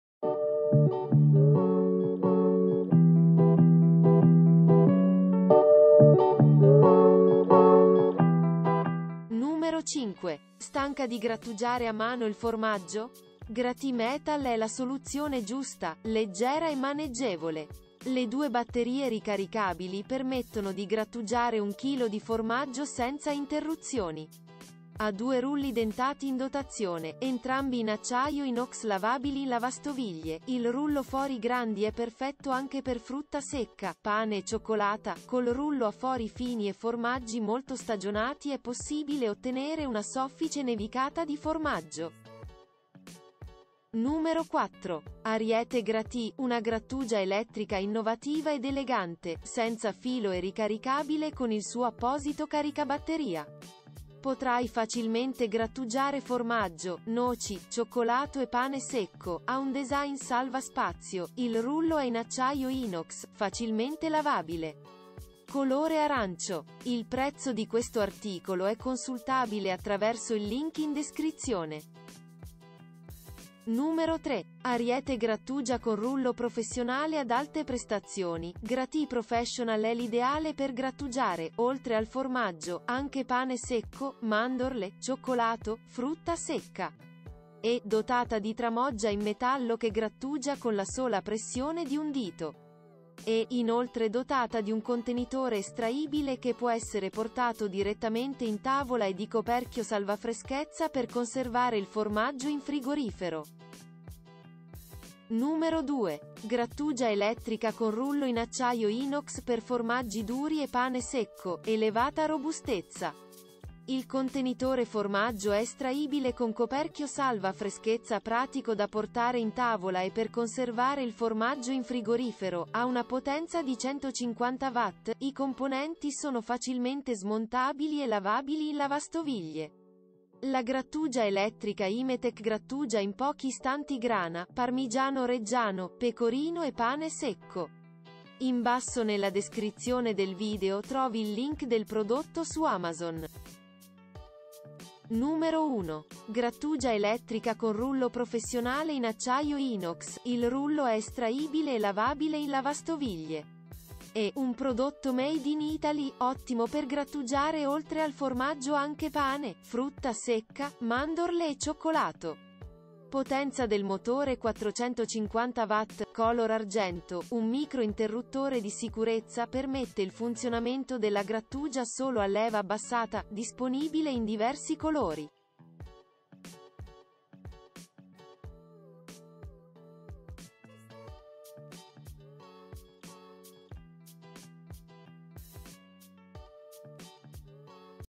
Le 5 migliori grattugie elettriche
1) RGV MAXI VIP 8G/S Grattug... »
2) Imetec GR1 1000 Grattugia... »
3) Ariete 448 Gratì Professi... »
4) Ariete 447 Gratì 2.0 - Gr... »
5) Ariete 44 Gratì Metal - G... »

✋ Ecco le 5 migliori grattugie elettriche, disponibili sul mercato.

00:00 - Intro
00:09 - 5) Ariete 44 Gratì Metal - Grattugia Elettrica Ricaricabile per...
00:43 - 4) Ariete 447 Gratì 2.0 - Grattugia Elettrica Ricaricabile Senz...
01:13 - 3) Ariete 448 Gratì Professional - Grattugia elettrica con rull...
01:45 - 2) Imetec GR1 1000 Grattugia Elettrica, Rullo in Acciaio Inox, ...
02:25 - 1) RGV MAXI VIP 8G/S Grattugia Elettrica con Rullo Professional...